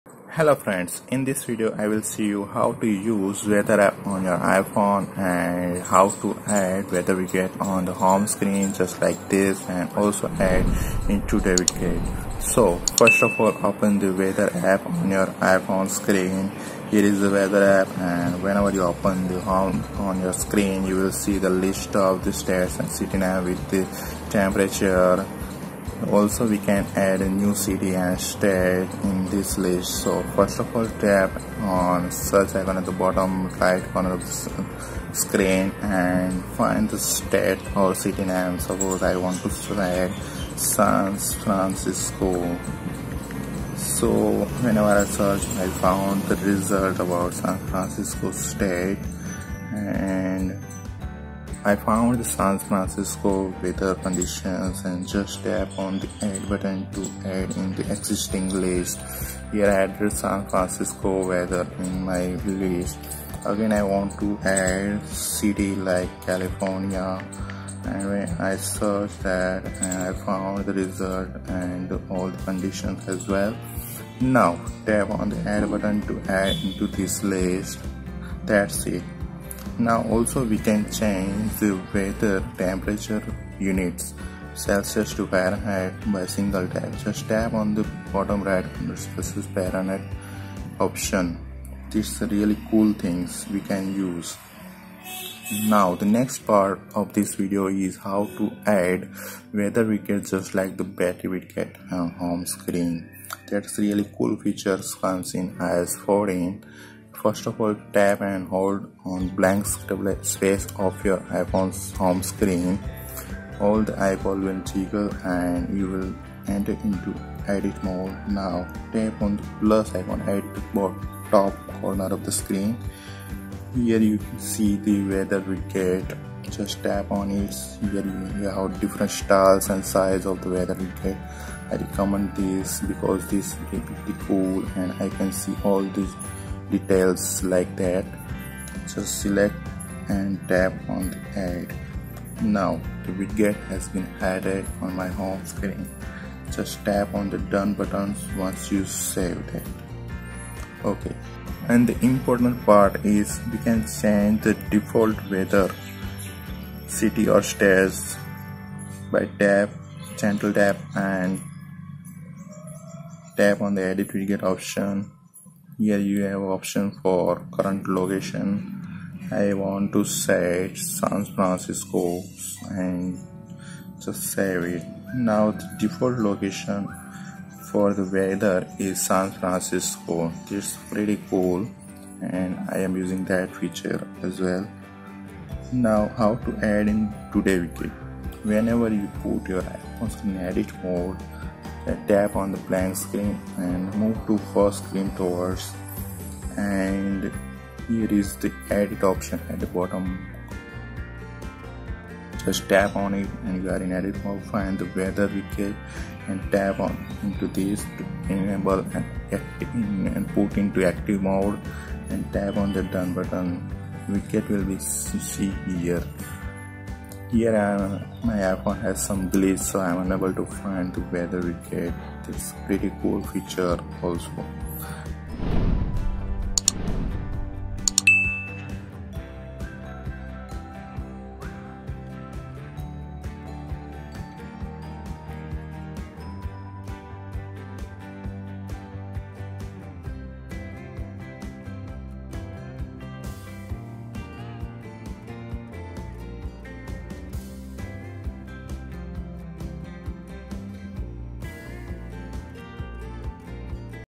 How to Add/Remove City/State in Weather App in iOS 14 on iPhone: Widget to Home Screen/Today Widget
Here in this video, I will see you about how to add or remove the city & state in the Weather iPhone Weather app. Also, Add Widget to the iPhone Home screen, Change the Default location on Weather Widget on iPhone, Rearrange Weather city and state list on the iPhone Weather app, Delete Data from Weather app on iPhone, add new Weather Data on iPhone Weather app. tested in iOS 14 on iPhone 11, 11 Pro Max, iPhone Xr, iPhone Xs Max, iPhone Xs, iPhone X, iPhone 8/8 Plus, iPhone 7/7 Plus, iPhone 6S/6S Plus, iPhone 6/6 Plus, iPhone 5S/5.

00:15 - How to use Weather App on iPhone
00:28 - Add a new city and State
01:40 - Add Weather Widget to Home screen
02:33 - Change Default Weather Location in Widget on Home screen

XX XX